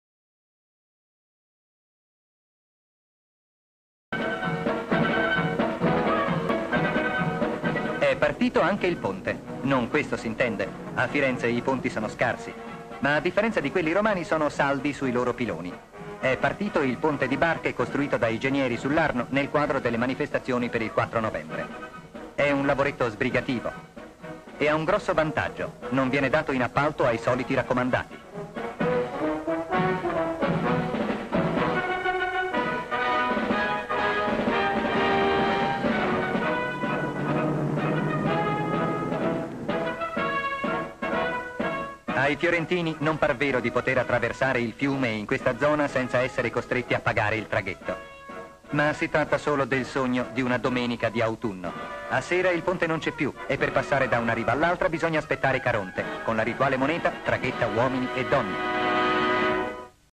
E' partito anche il ponte.
Gli uomini dell'Arma del Genio in occasione della Festa delle Forze Armate costruiscono, servendosi di elementi prefabbricati e facilmente componibili, un ponte galleggiante tra le due rive dell'Arno.
   Descrizione sequenze:automobili attraversano un ponte ; ponte sull'Arno ; folla assiste alla manifestazione ; un uomo dell'Arma del Genio spara un colpo di pistola in aria ; uomini del Genio s bordo di barche sull'Arno ; uomini sull'imbarcazione lanciano a riva una corda ; gli uomini del Genio accostano tra loro i componenti del ponte prefabbricato e galleggiante ; un carro armato attraversa il ponte di legno prefabbricato e che poggia su gommoni galleggianti ; folla disciplinata attraversa il ponte ; due cani a riva ; un gruppo di uomini a bordo di una barca a remi attraversano il fiume da una sponda all'altra ;